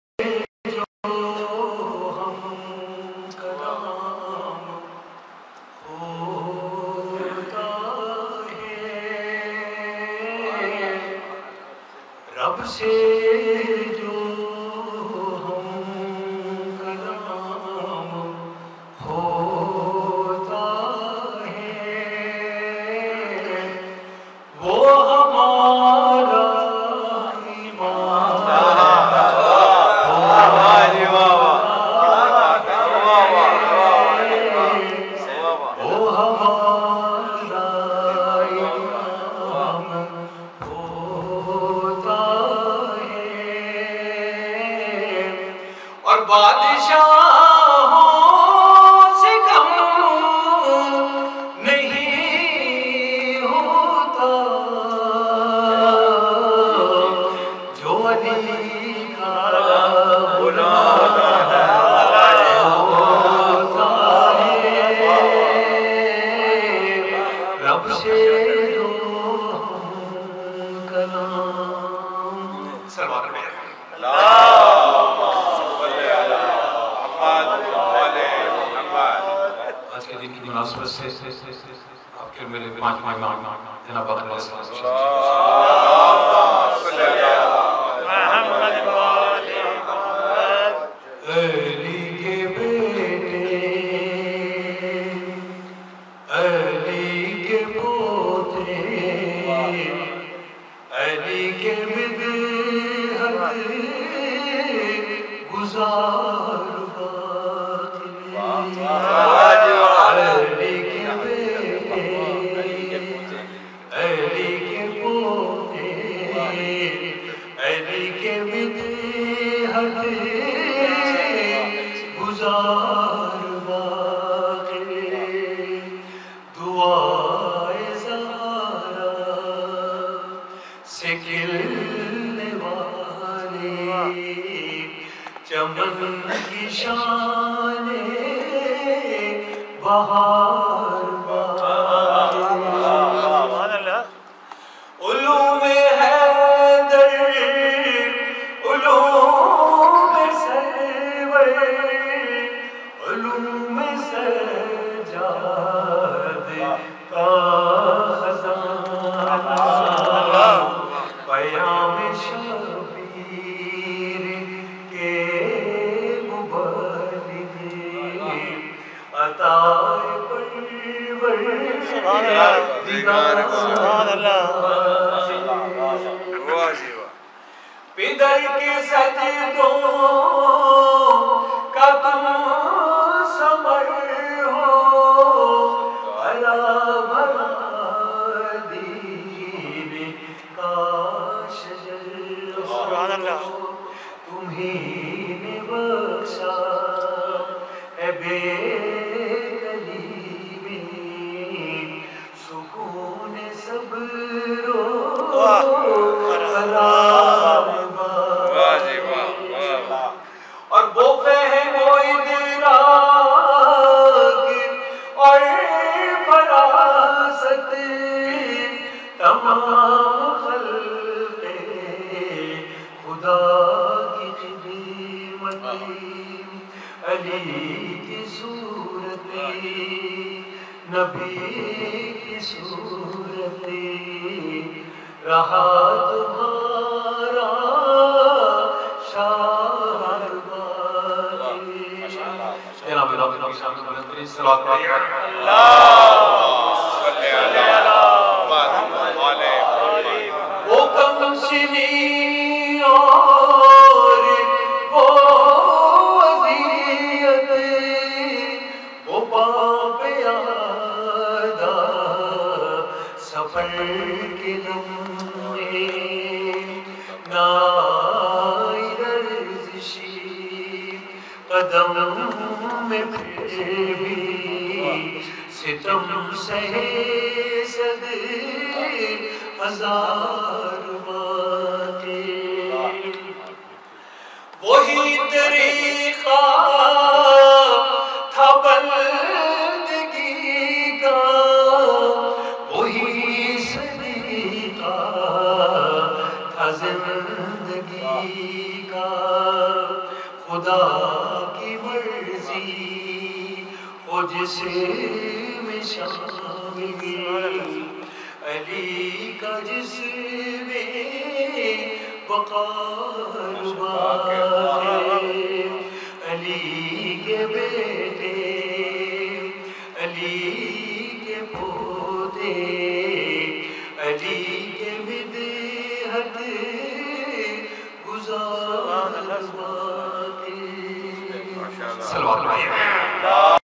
Syed Raza Ali Kazmi 010514 1 Mehfil Hadise Kisa Res Janab Wajid Bukhary Islamabad
تاریخ : 01مئی 2014 بمطابق 01 رجب المرجب 1435 ہجریٍ
550 محفل حدیث کساء
میزبان واجد بخاری صاحب
مقام : رہائیش جناب واجد بخاری ایف ٹین اسلام آباد
ویڈیو پیشکش : سید شمشاد حیدر